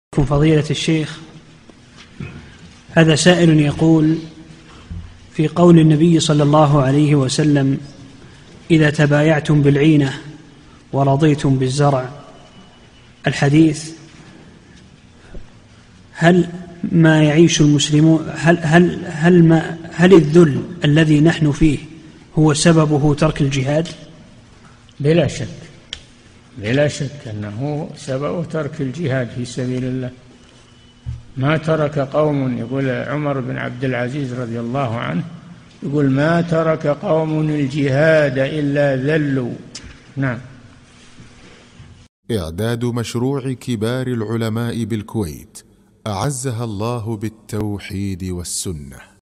[2884 -3022] هل الذل الذي نحن فيه سببه ترك الجهاد؟ - الشيخ صالح الفوزان
-"[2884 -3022] السؤال: في قول النبي ﷺ: (إذا تبايعتم بالعينة ورضيتم بالزرع) الحديث هل الذل الذي نحن فيه هو سببه ترك الجهاد؟
الجَوَابُ: بلا شك، بلا شك أنه سببه ترك الجهاد في سبيل الله، يقول عمر ابن عبد العزيز رضي الله عنه: ما ترك قومٌ الجهاد إلا ذلوا.
للاشتراك في قناة الشيخ صالح الفوزان

للاستفسار والإقتراحات والدعم
رئيس الفريق
لمشروع كبار العلماء